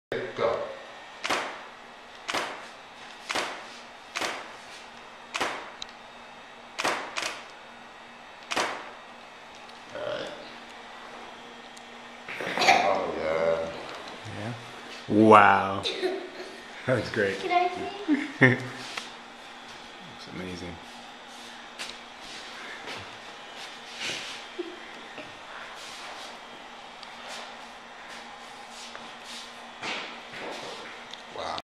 Lexi's Photoshoot With Petey Wheatstraw
Qick clip from my daughters photoshoot with Photographer Petey Wheatstraw. More to come. Recorded on June 18, 2011 using a Flip Video camera.